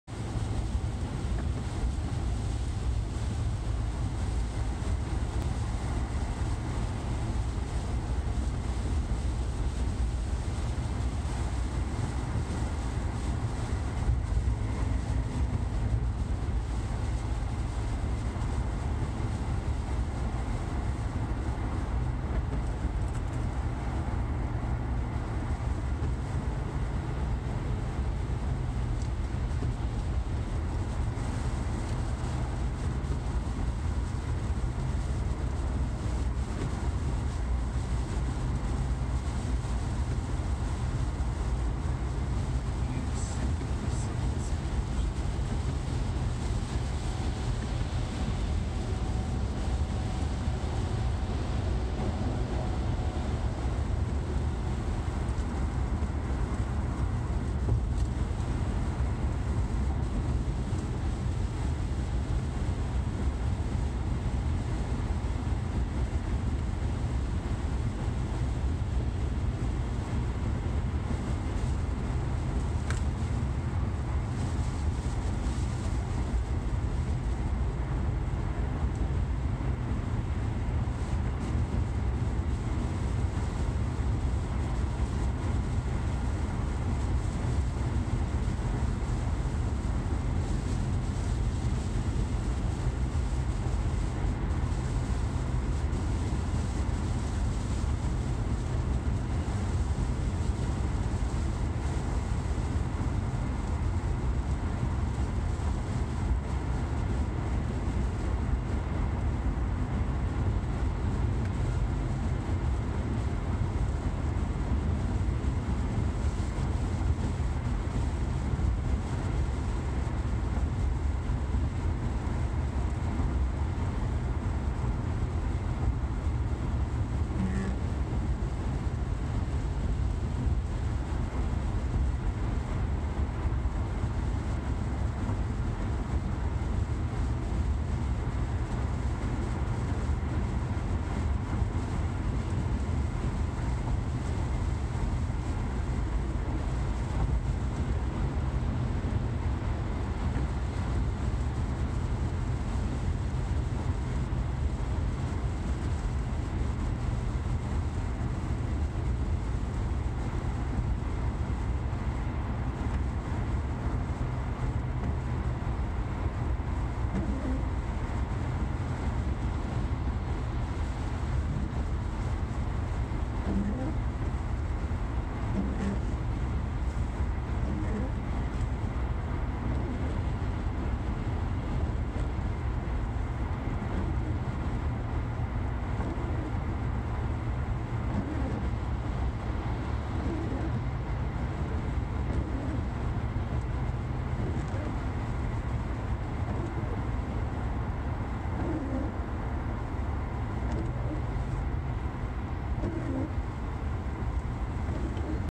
Ohio Drive: ASMR Rainy eastbound drive on Interstate 70 outside Columbus in a Crown Victoria
Relax to the gentle rain on this drive outside Columbus, Ohio. Summer is not so bad when there are rains to cool everything off. Experience the ASMR effect with the rains of the Crown Victoria heading east on Interstate 70.